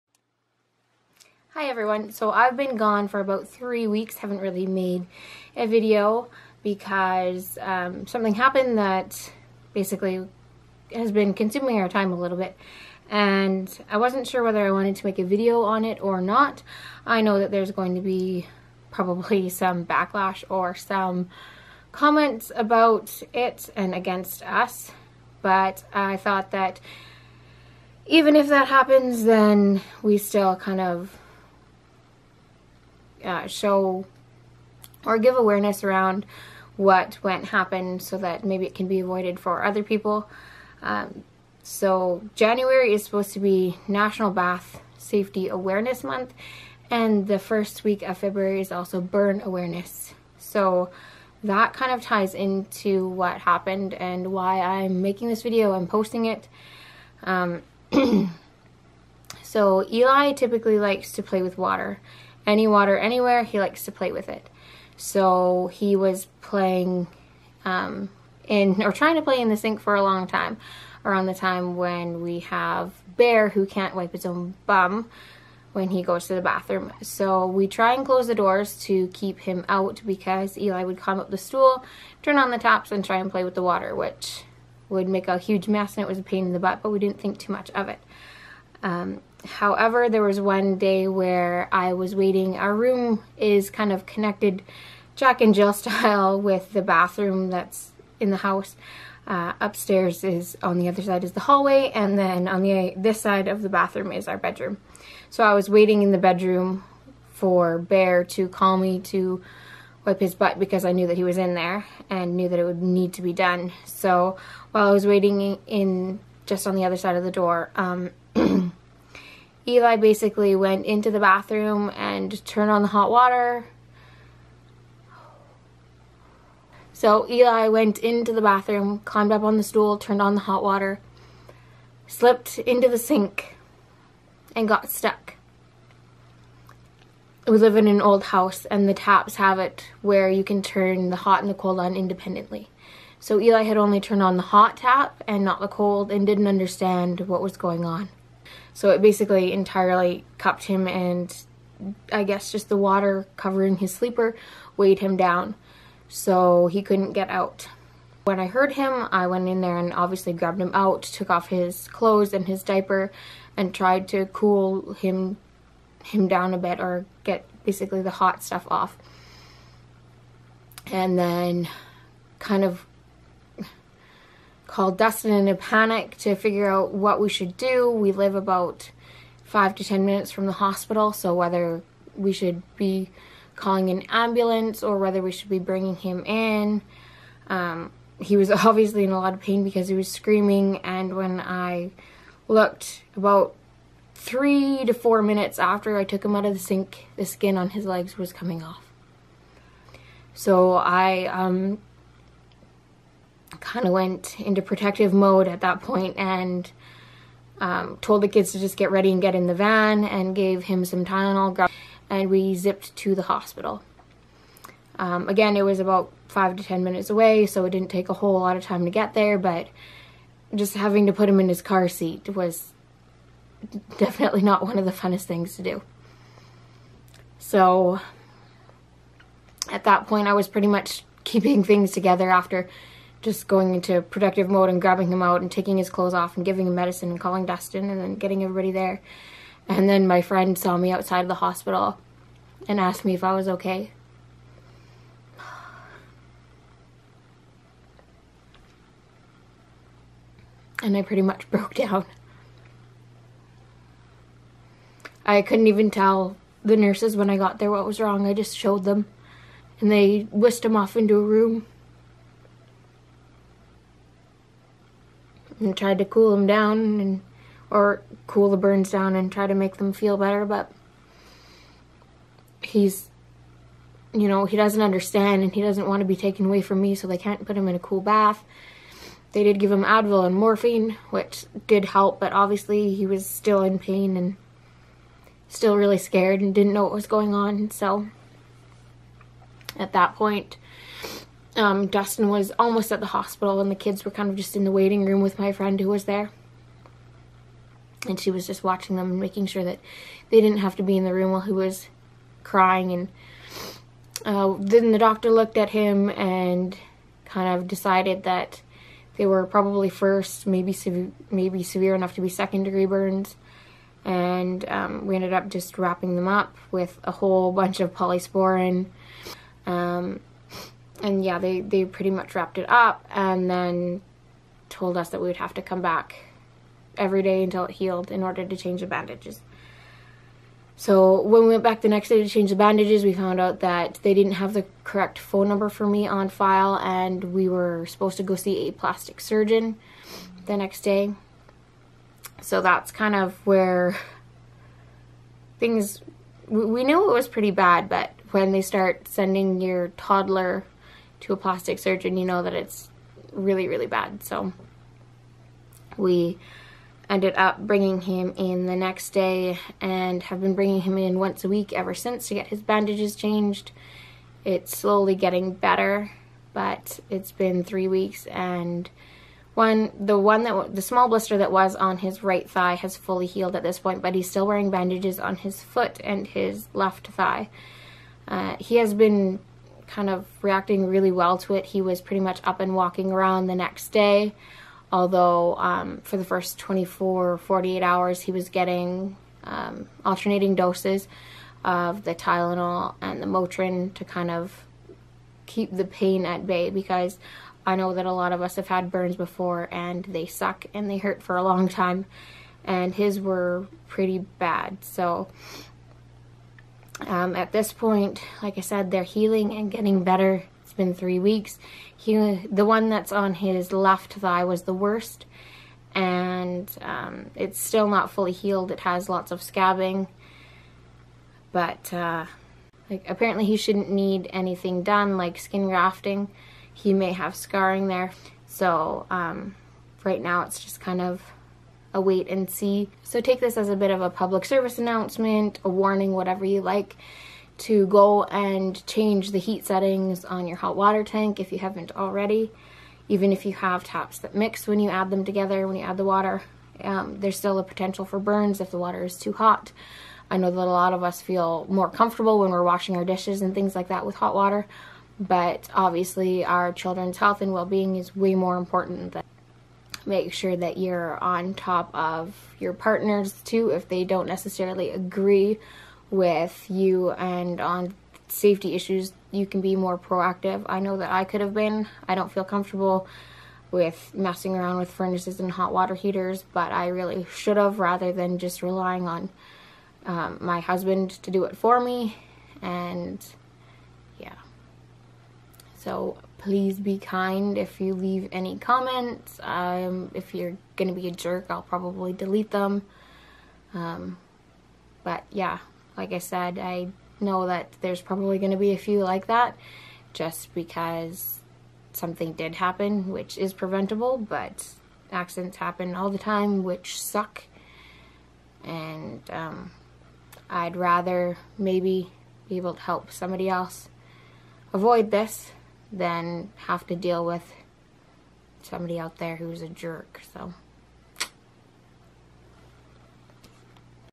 Why My 16 Month Old Has To See A Plastic Surgeon | Warning Graphic Description
No graphic images etc displayed | Graphic description!

This is why we have taken a bit of a break from posting.